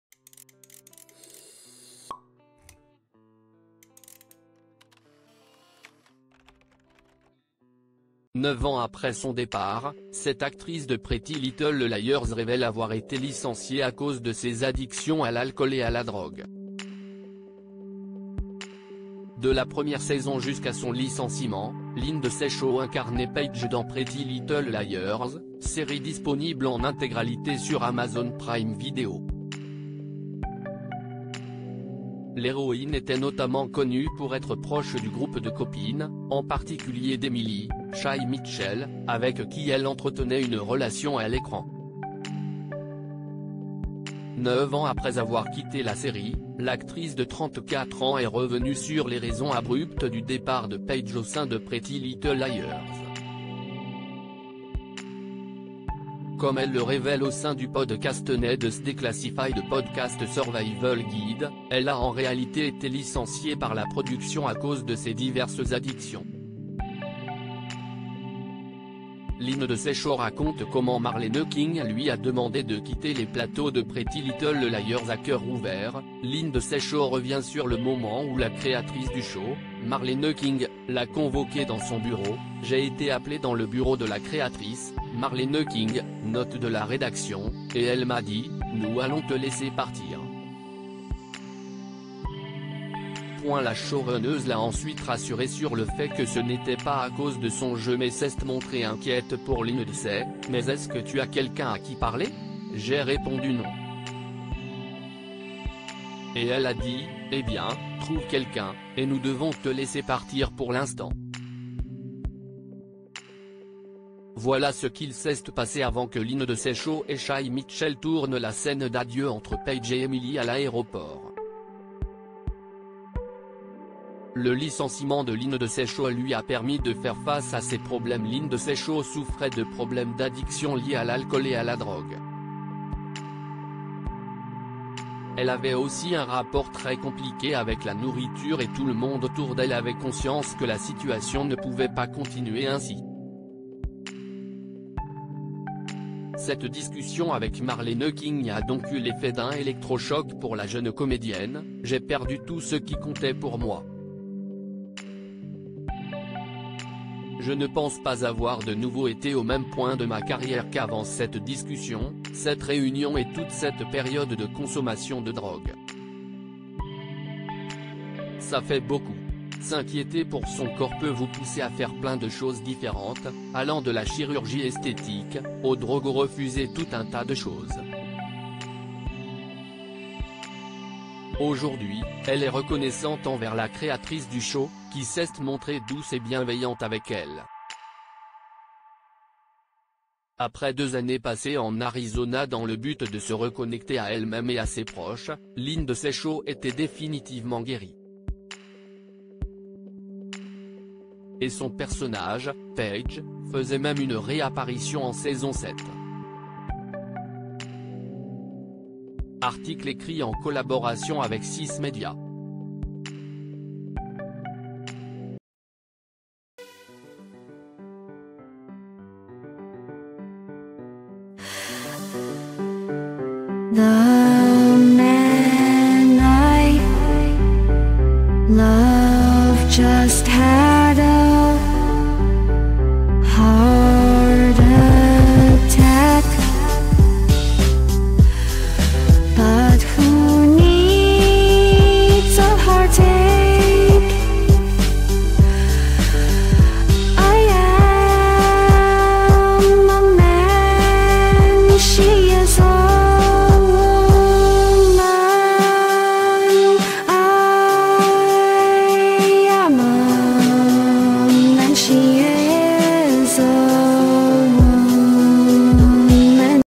Pretty Little Liars : 9 ans plus tard, cette actrice révèle avoir été virée à cause de ses addiction
Pretty Little Liars : 9 ans plus tard, cette actrice révèle avoir été virée à cause de ses addictions